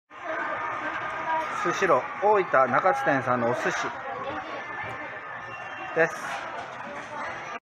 お食事処：スシロー 大分中津店188 ～小林消防設備 福岡県豊前市 全類消防設備士 第二種電気工事士 経営学修士～
動画は、大分県中津市にあるスシロー 大分中津店さんのお寿司です。
スシローさんは、メニューが豊富です。